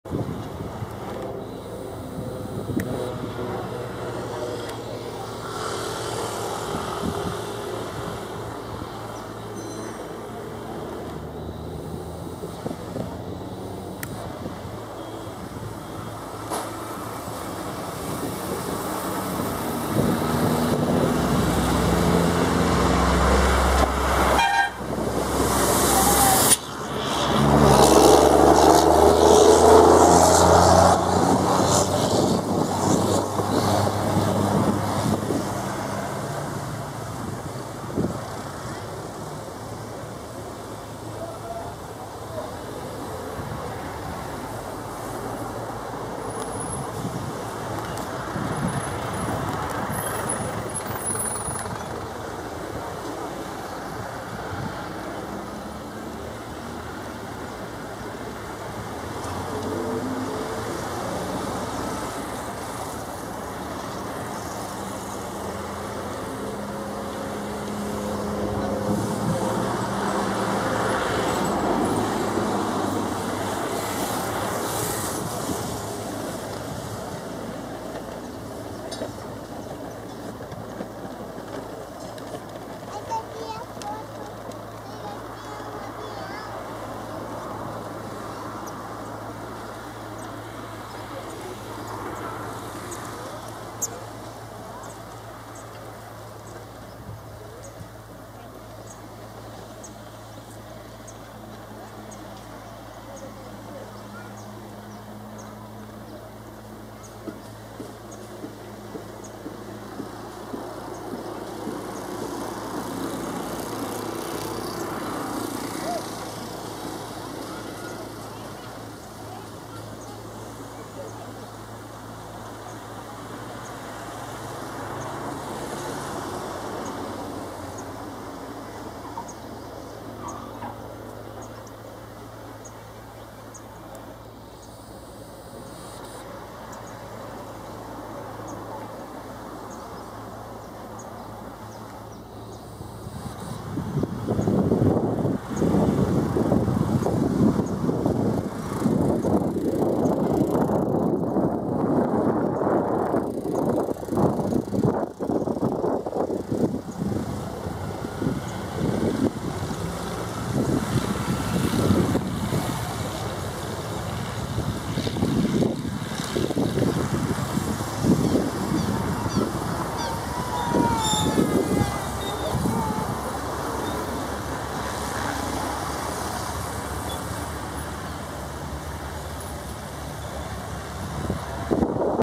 Beechcraft Baron 58 Aproximação Perna do Vento e Pouso em Angra dos Reis RJ
Beechcraft Baron 58 PT-LZW Pousando em SDAG